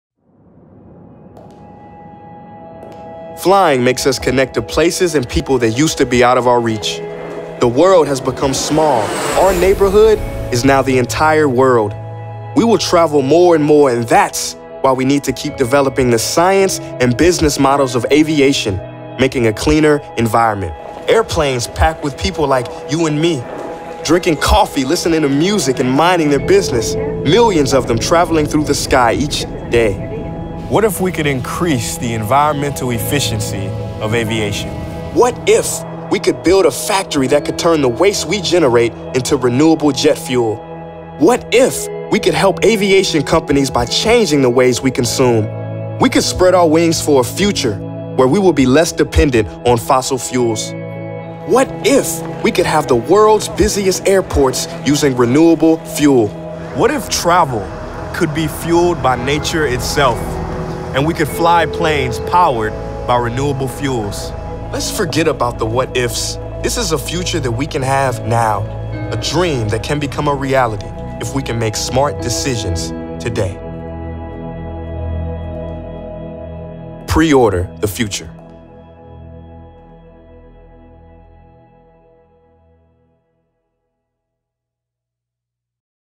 Prince Ea: What if we could fly sustainably?
Prince Ea: How can Neste’s renewable energy make flying more sustainable?